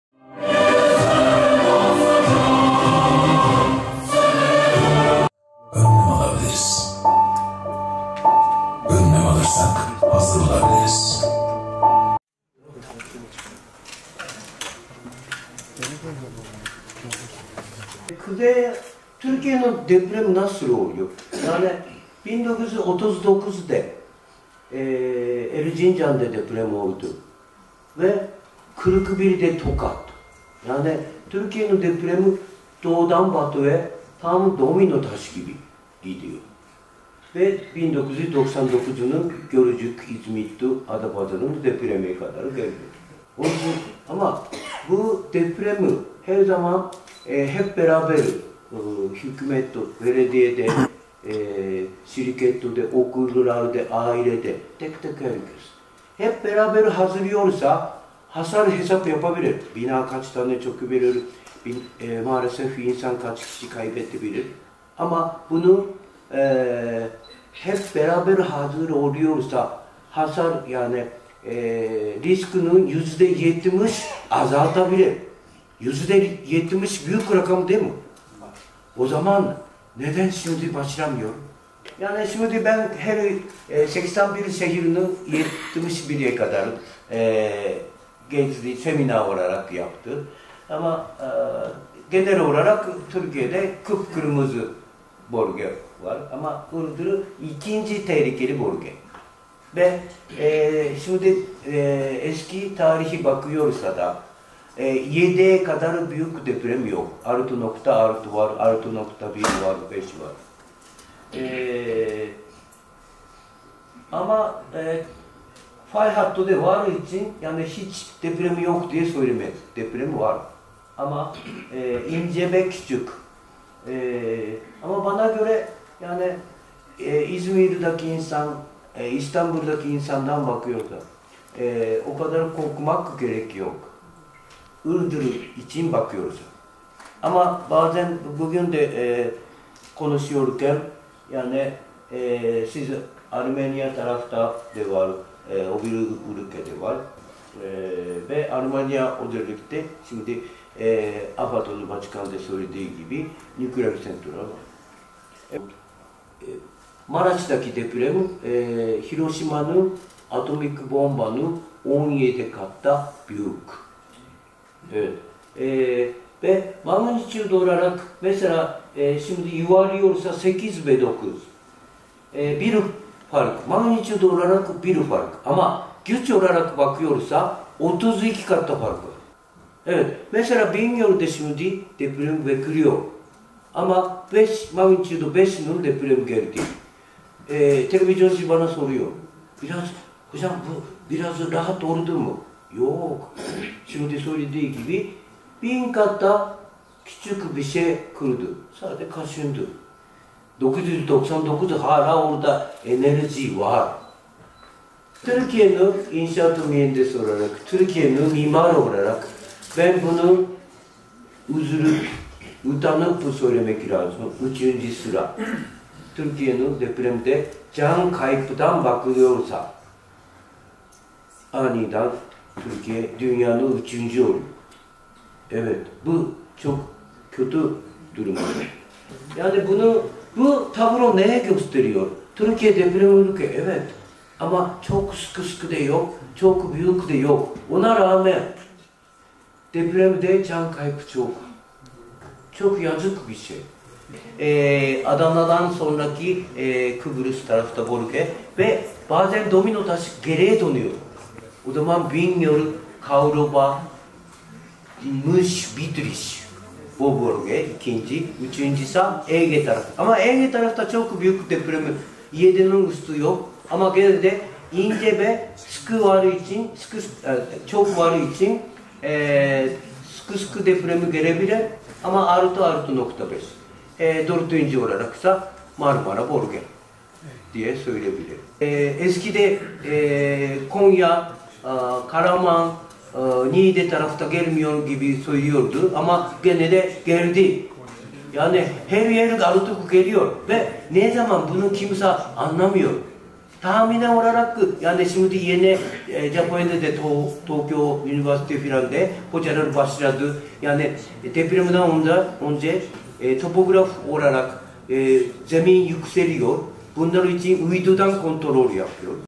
Deprem Uzmanı Moriwaki'den Türkiye'ye Uyarı: Bingöl, Karlıova, Muş ve Bitlis Dikkatli Olsun!
Yoshinori Moriwaki, Türkiye'deki depremler ile ilgili önemli bilgiler verdi. Konferansta, Moriwaki, Türkiye'nin deprem kuşağında yer alması ve bu nedenle sık sık depremlerle yüzleşmesi gerçeğine dikkat çekti.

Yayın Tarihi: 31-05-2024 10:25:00